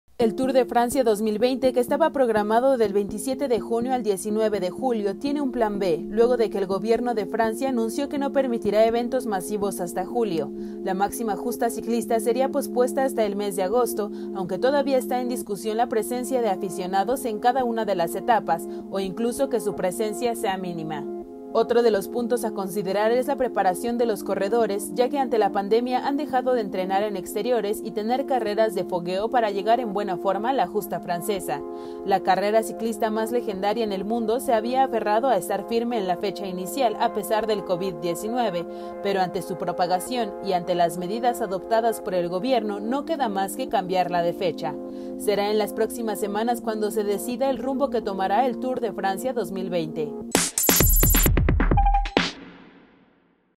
Tour de Francia 2020 tiene plan B para agosto
El Tour de Francia 2020 que estaba programado del 27 de junio al 19 de julio, tiene un plan B, luego que el gobierno de Francia anunció que no permitirá eventos masivos hasta julio.